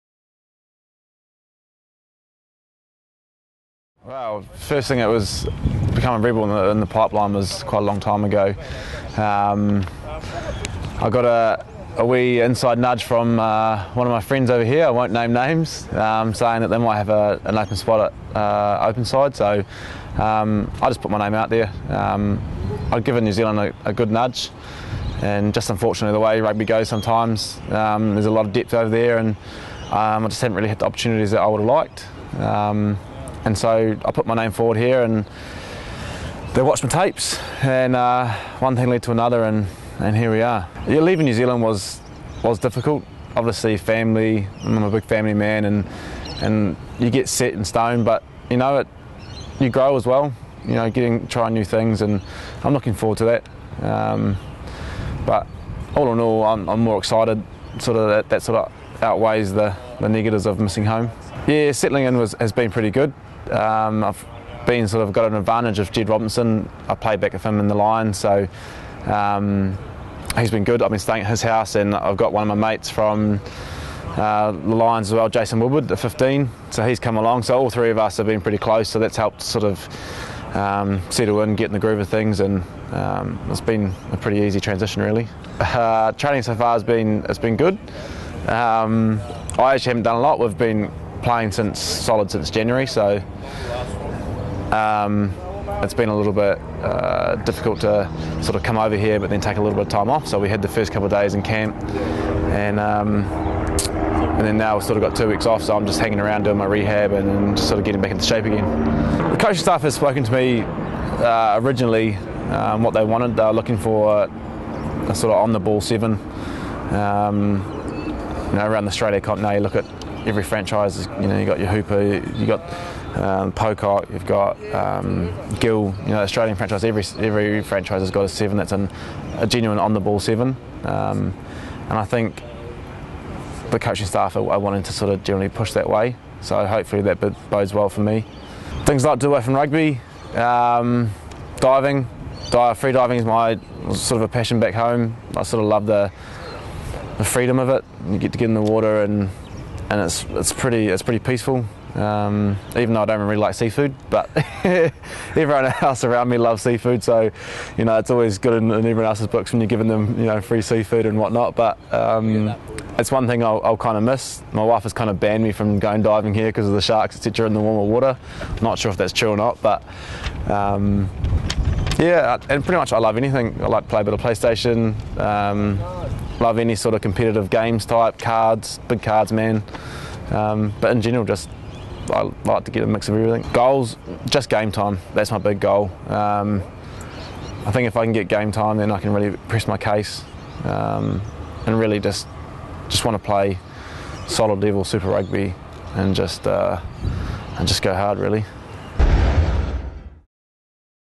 Introducing new RaboDirect Rebels recruit Scott Fuglistaller
Kiwi openside flanker Scott Fuglistaller joins the RaboDirect Rebels for the 2013 season with one goal in mind, to cement a spot in the starting 22 in the number seven jersey.

Fuglistaller has spent the past few seasons playing for the Wellington Lions in the local New Zealand competition, and was part of the Wellington Hurricanes Wider Training Group.

He was drafted into the Otago Highlanders Super Rugby squad in 2012 to cover for injuries, and after viewing his tapes, the Rebels coaching staff decided Fuglistaller was definitely worth signing.